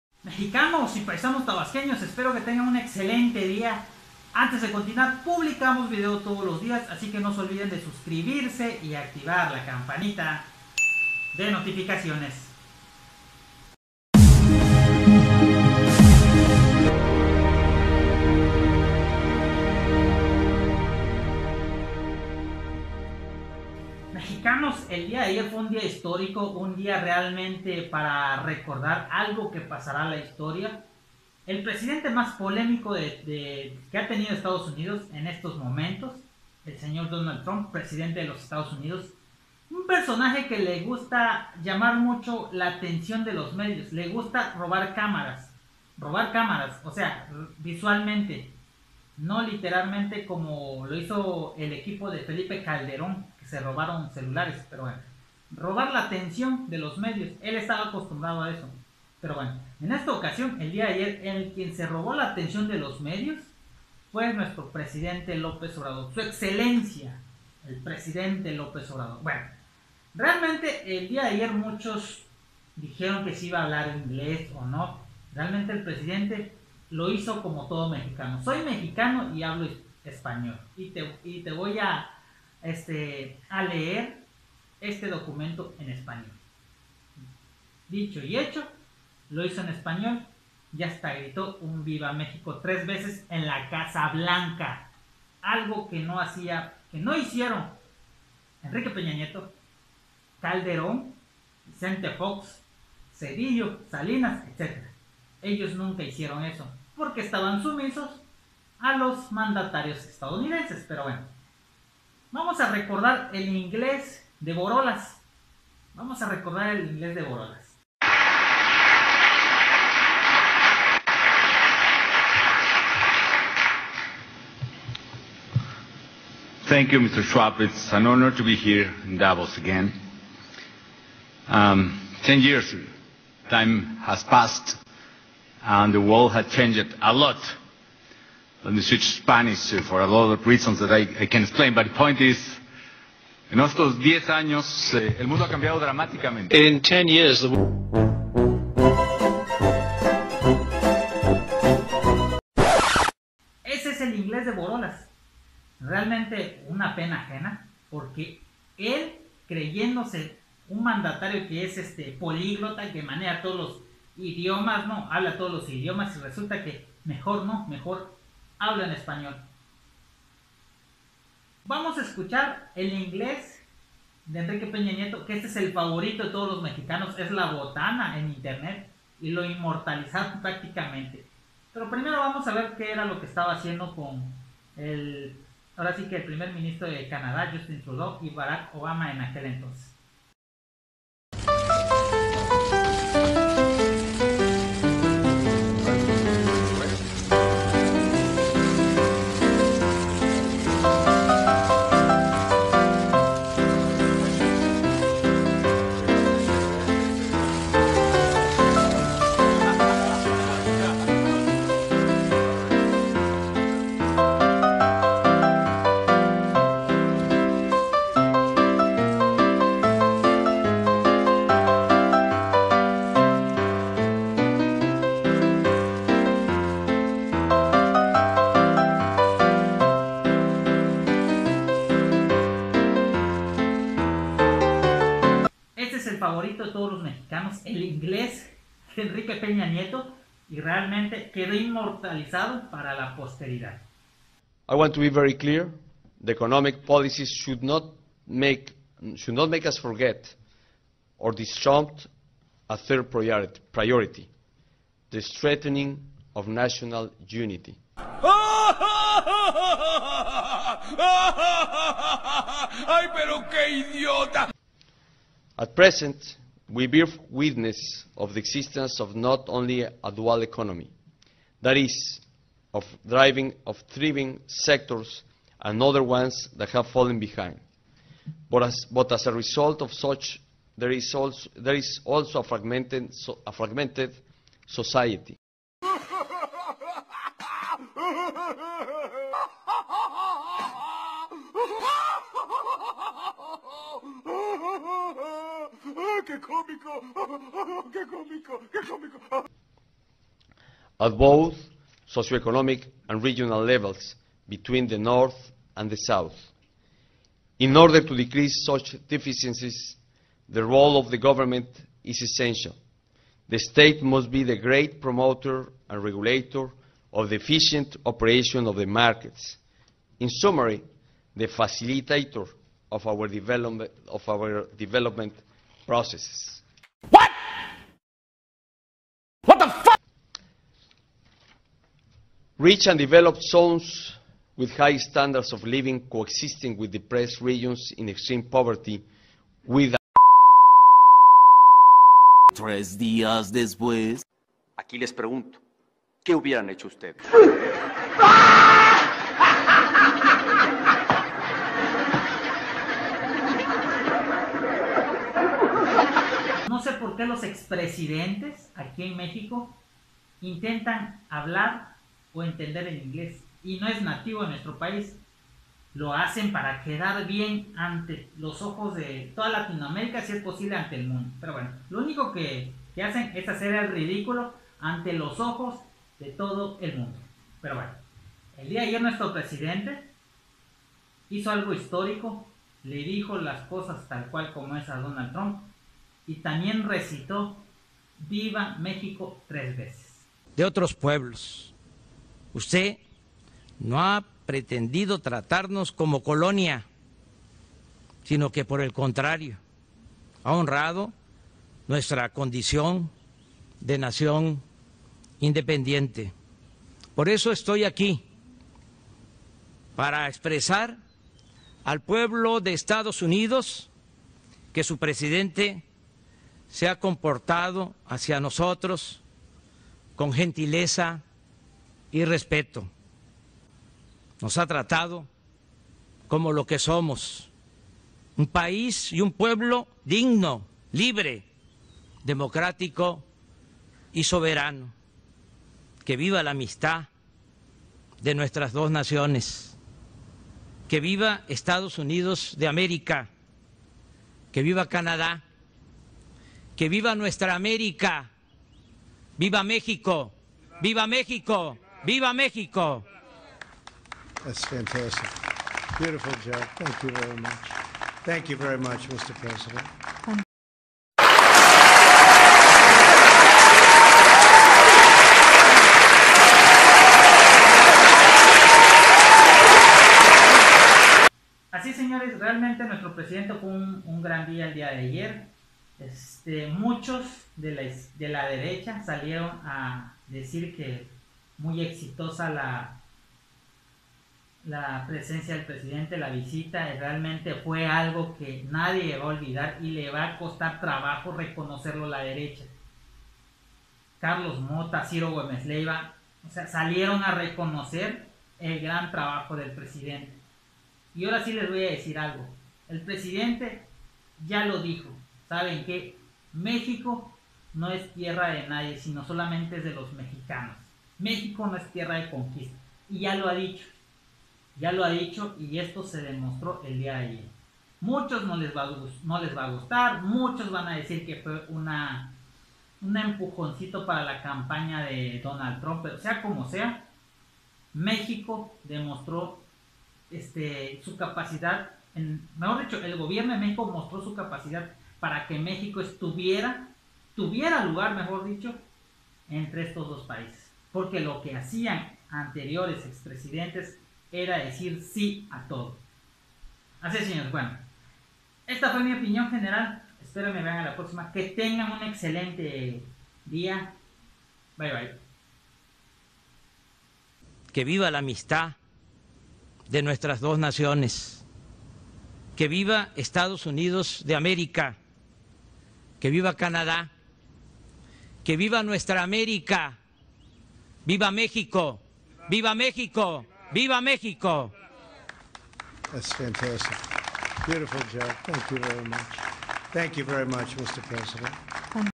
HISTORICO LA VISITA DE SU EXCELENCIA LOPEZ OBRADOR
Mexicanos y Paisanos Tabasqueños... Nuestra excelencia el presidente Andres Manuel Lopez Obrador hizo historia, VIVA MEXICO 3 VECES EN LA CARA DE TRUMP Y EN LA CASA BALNCA... Saludos.